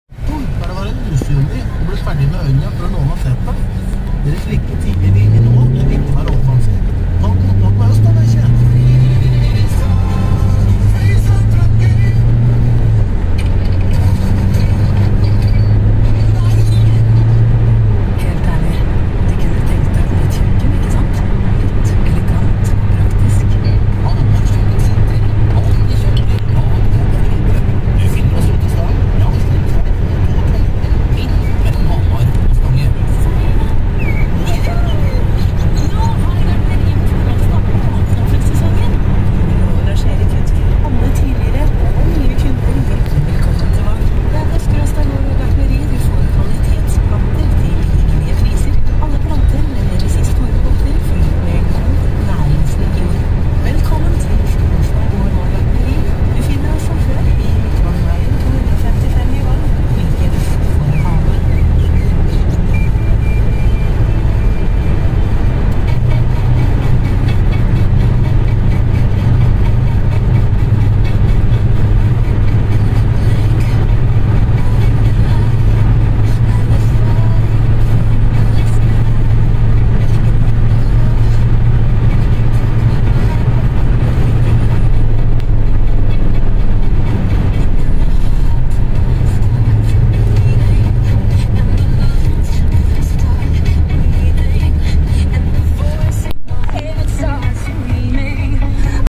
2005 Toyota Corolla e12 1.4 d4d remaped. 1.4 bar boost
2005 Toyota Corolla e12 1.4 d4d remaped. 1.4 bar boost and 1.5 peak boost